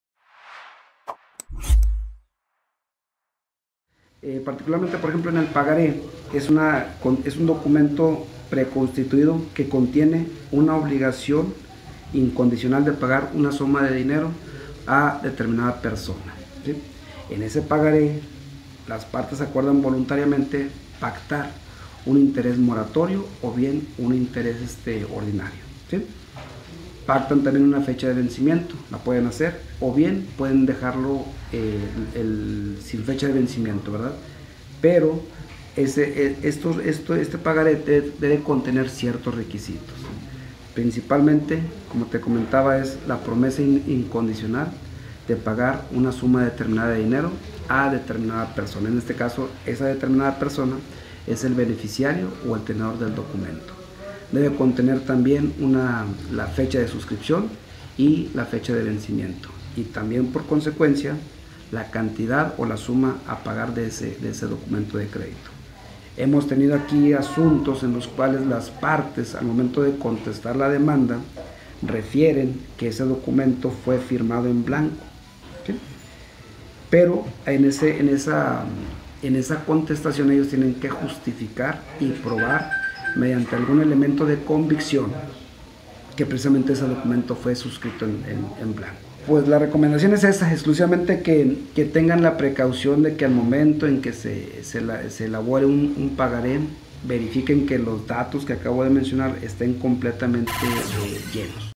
RECOMIENDAN SER CUIDADOSOS AL FIRMAR PAGARÉS
El juez de Primera Instancia de lo Civil en el Distrito Judicial de Acuña, Modesto Corpus González, mencionó que se debe de ser cuidadoso al momento de llenar y firmar un pagaré, para evitar problemas legales posteriores.